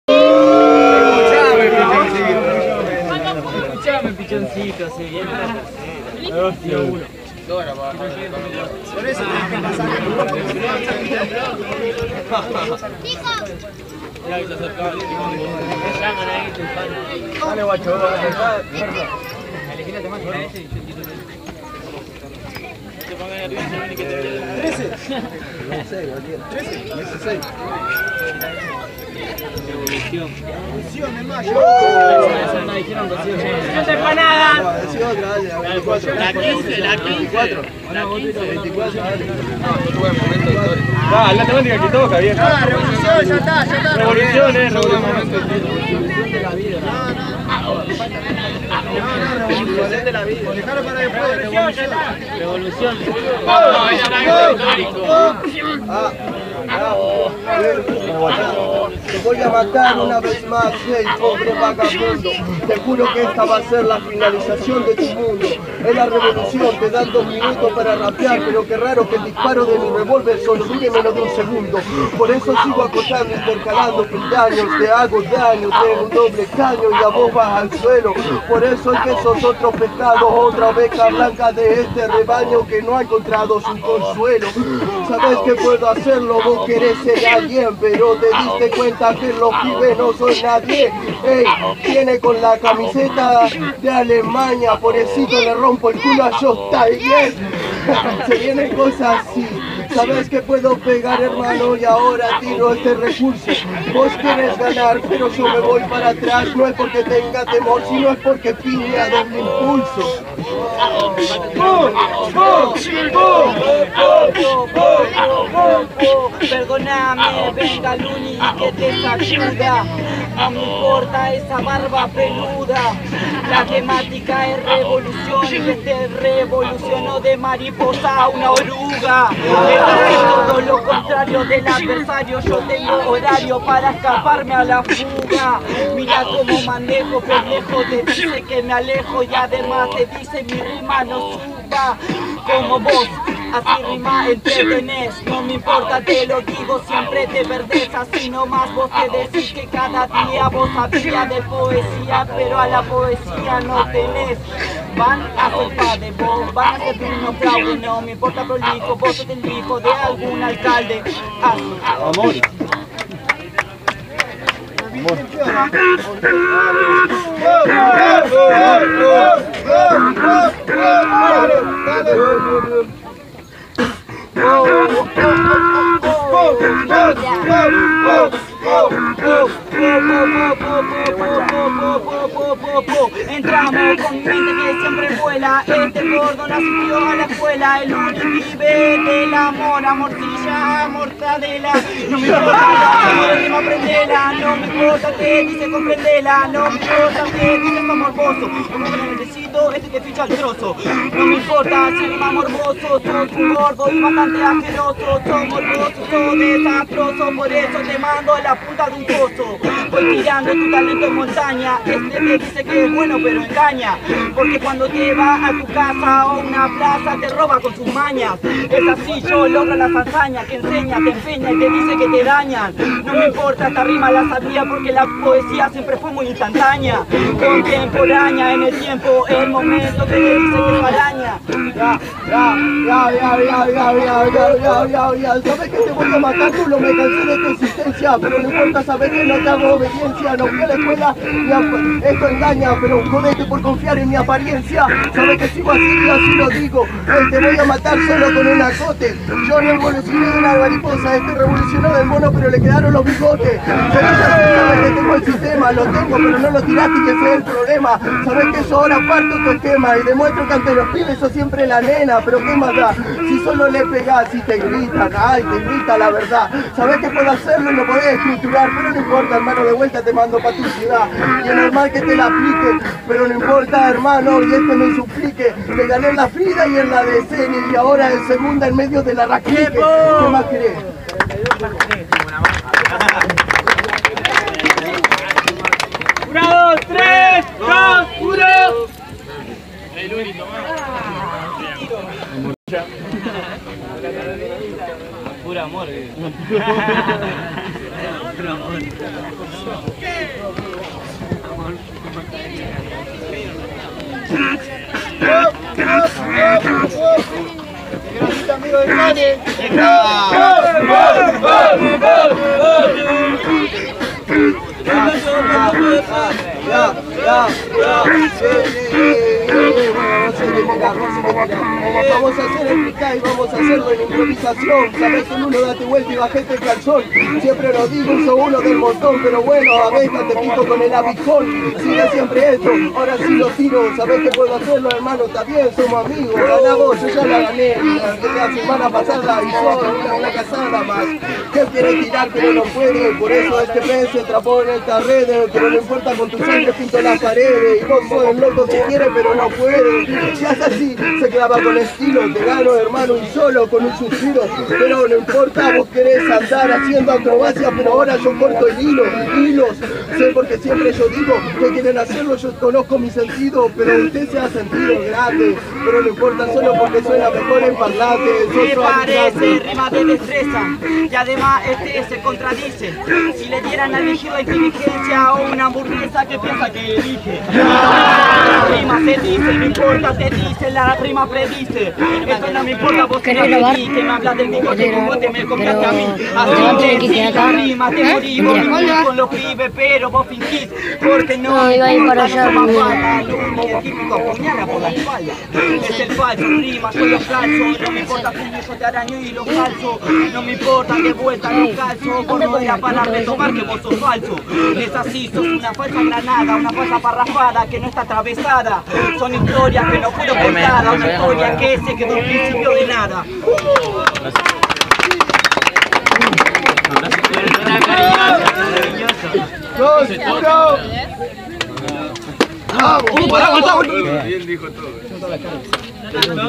Luni vs Lulo 4to Rap Clique 6ta Edicion
Bueno , Salio la sexta edición de Rap Clique , se hace en la bajada del puente carretero (Santo Tomé)
Domingo 31 a las 19:00
1 vs 1 CON TEMÁTICA , Si hay replica es libre (SIN TEMÁTICA).
Compiten todos..

Temática: Revolución

Ganador: Lulo